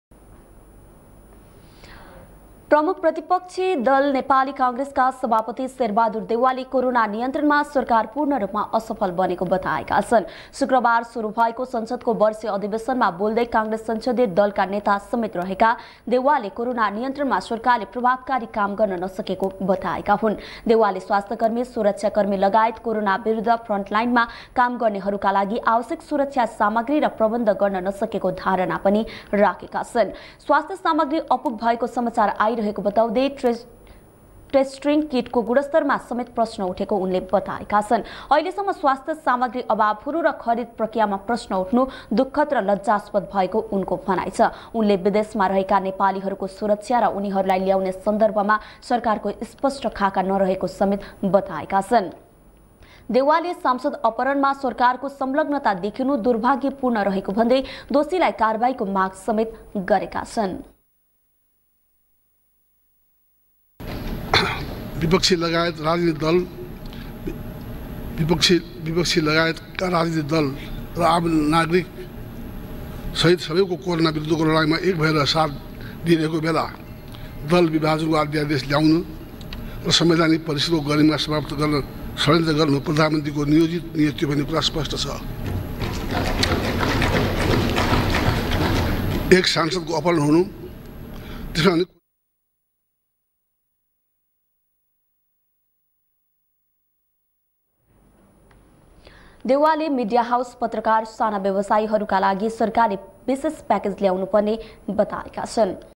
देउवा भन्छन् कोरोना नियन्त्रणमा सरकार पूर्णरुपमा असफल
कोरोना नियन्त्रणमा सरकार पूर्णरुपमा असफल देउवा भन्छन् स्वास्थ्य सामाग्री अभाव हुनु र खरिद प्रक्रियामा प्रश्न उठ्नु दुस्खद र लज्जास्पद